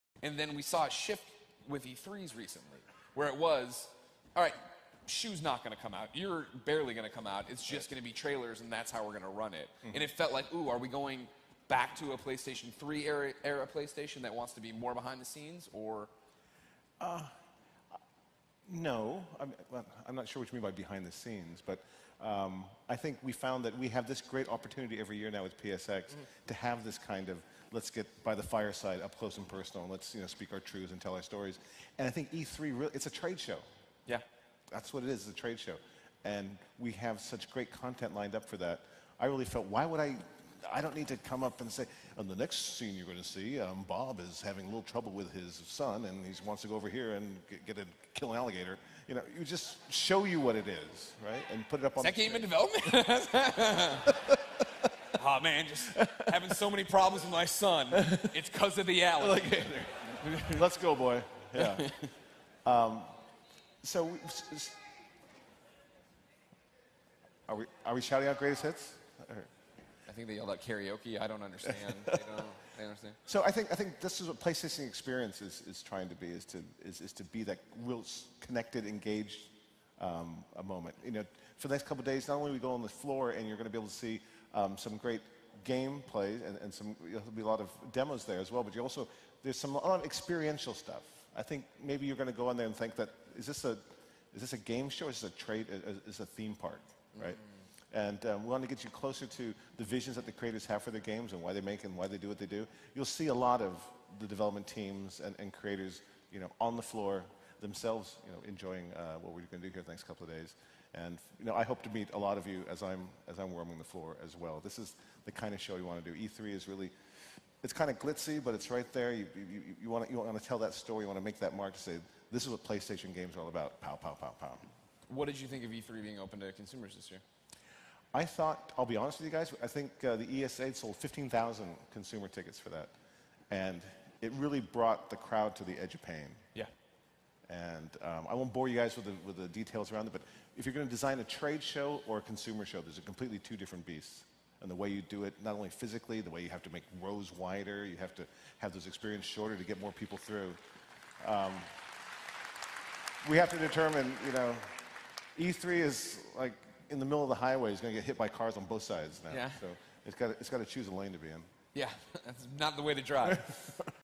Shawn Layden Hinted That Sony Was Done with E3 a Year Ago
This is an interesting video to watch in hindsight, now that Sony has announced it won’t be attending E3 2019 at all. Kyle Bosman from Easy Allies was the first to bring it up, but it’s since been shared around the web, and it does foreshadow more recent news. The clip is taken from last year’s PlayStation Experience, and you get the sense that Worldwide Studios chairman Shawn Layden is very frustrated with the convention and what it’s become.

In the video, Kinda Funny co-founder Greg Miller touches upon the changes to Sony’s E3 format, and how recent press conferences have effectively been trailer montages. Layden explains that there are better opportunities for the suits to speak, but he also hints that he’s not entirely sure what E3 is anymore; he suggests that by straddling the line between the public and trade the show is “in the middle of the highway and it’s going to get hit by cars on both sides”.

It’s fascinating watching this video with the benefit of hindsight as you can see there’s some pent-up frustration from Layden. The fact that he talks about the company wanting to build “experiences” around its demos probably goes some way towards explaining E3 2018’s presentation, where the manufacturer moved the press between “themed” rooms based on its blockbuster games like The Last of Us: Part II.

It also lends credence to the idea that Sony’s going to do something completely different instead of E3 next year. Our guess is that it’s probably going to run a series of fan events, and speak directly to its players rather than the press or trade. It could go one of two ways: vacating that all-important E3 period is risky, but it’s perfectly plausible that it may stumble upon something that’s even more engaging for all of us.